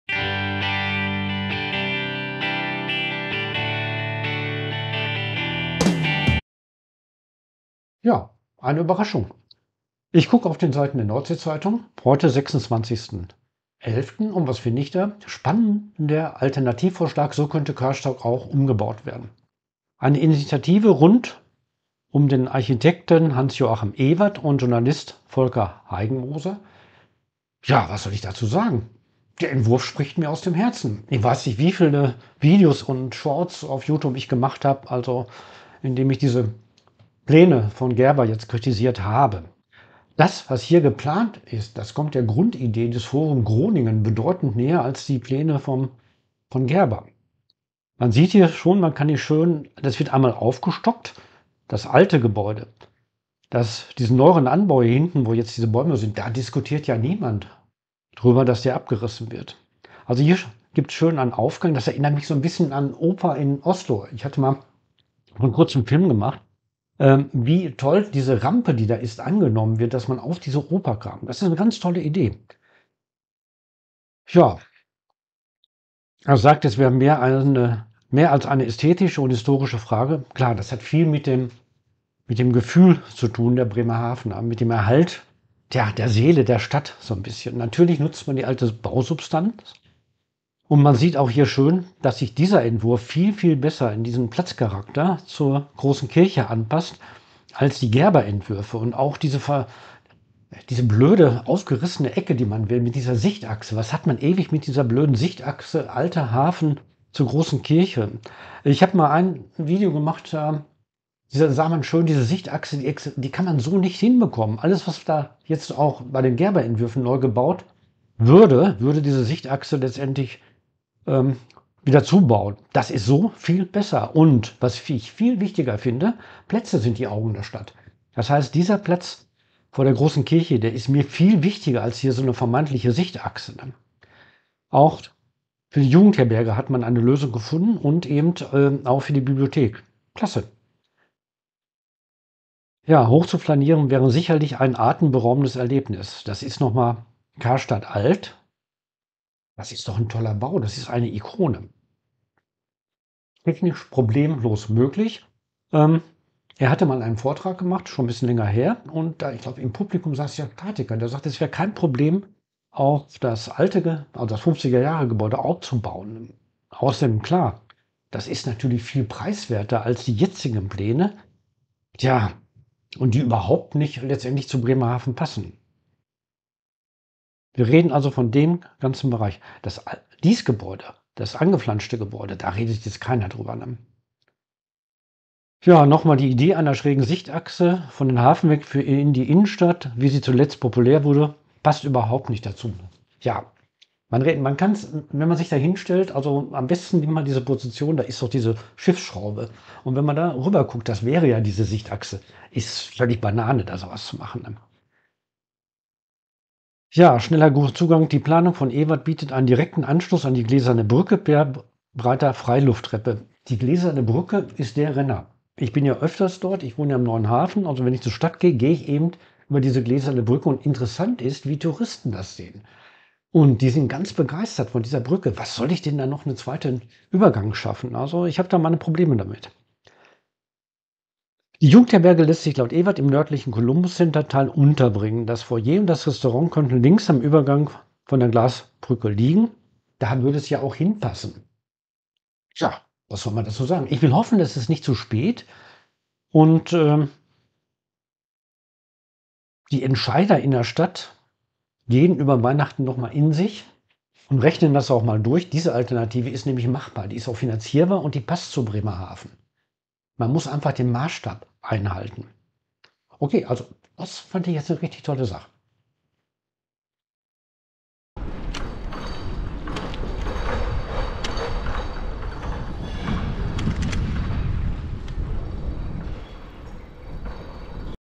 Die Nordsee Zeitung: Alternative zum Karstadt Abriß
Es gibt eine Alternative zum Abriß des Karstadt Gebäudes, die nah an der Stadt, nah an den Menschen, die hier leben und nach an der Zeit ist, in der gelten sollte: Umbau statt Abriß!
Jens Gehrke berichtet von einem alternativen Vorschlag.